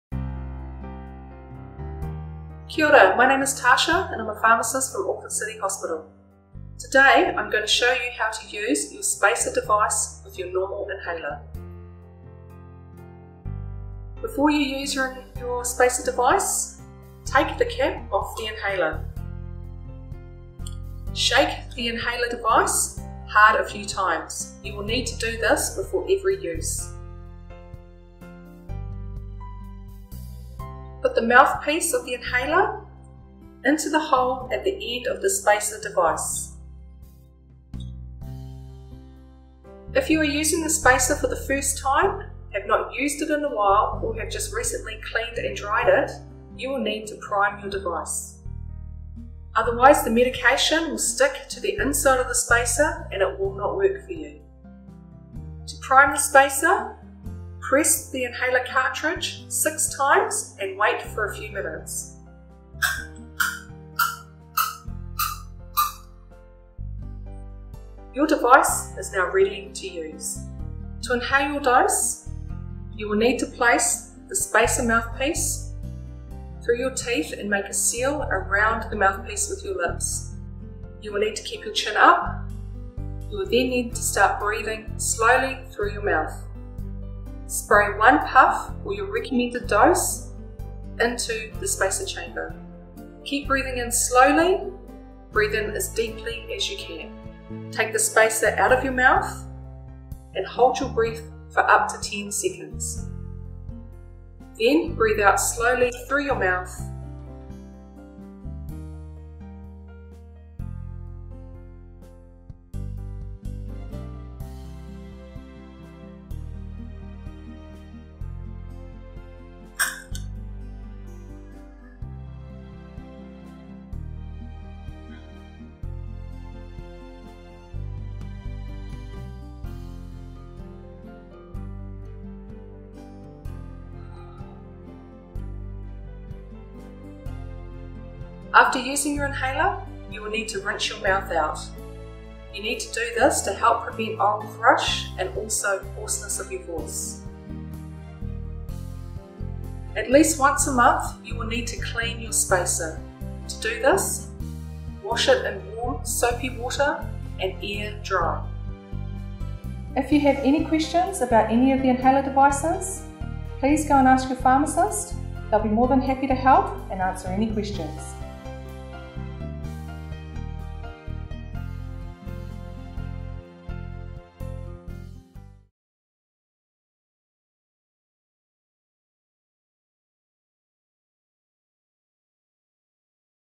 How to use your Spacer device
This easy-to-follow video shows you how to use your spacer device with your inhaler, including how to prime the spacer, an inhaler demonstration and tips on how to clean your spacer.

For more information about this spacer and other health topics,